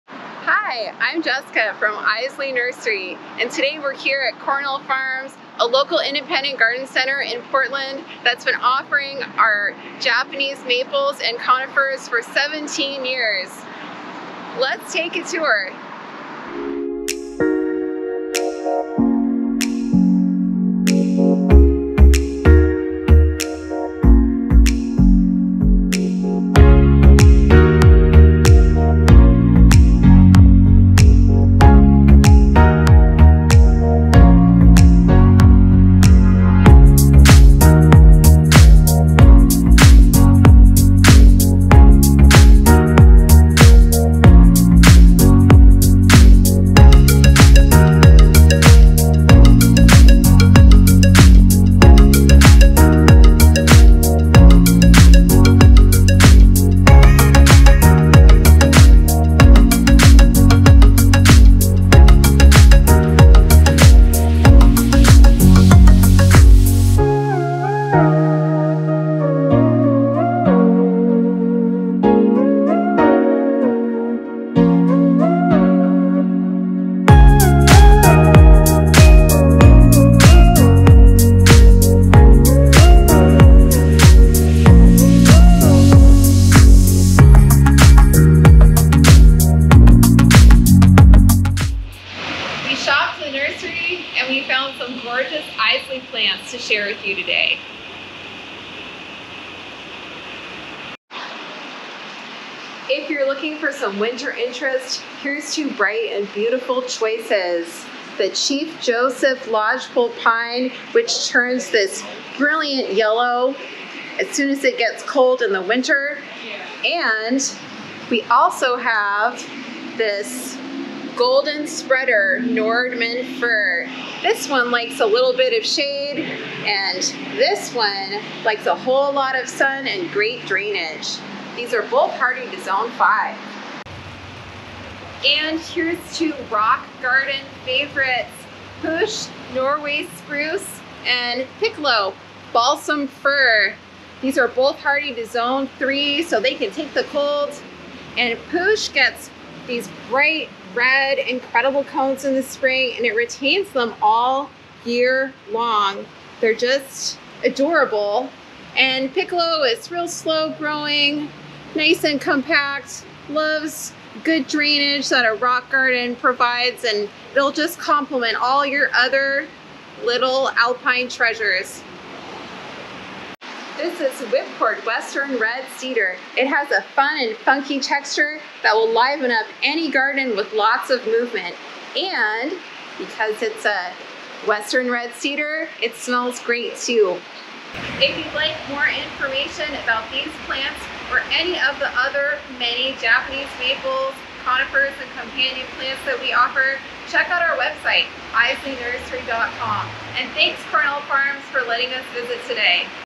A visit to Cornell Farms
Join us on a visit to Cornell Farms, a local independent garden center that has been offering Iseli plants for 17 years. Jessica will also highlight a few Iseli conifers she found in the nursery.